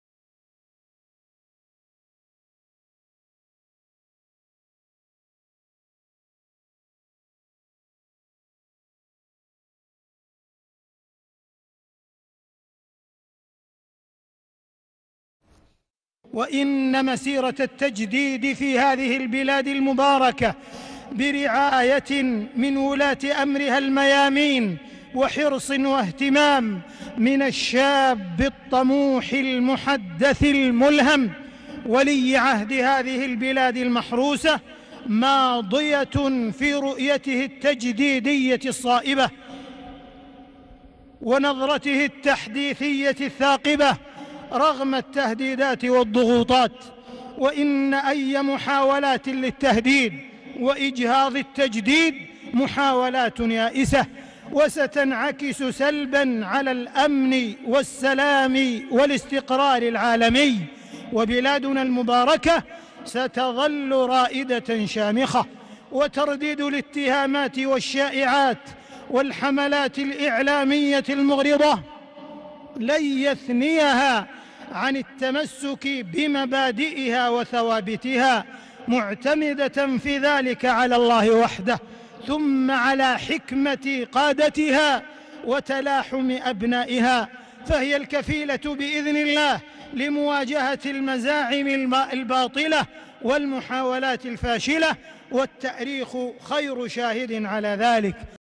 Mecca Imam defends Saudi Crown Prince, calls him 'divinely inspired'
Description: (From the archives)

Source: Al Saudiya (YouTube)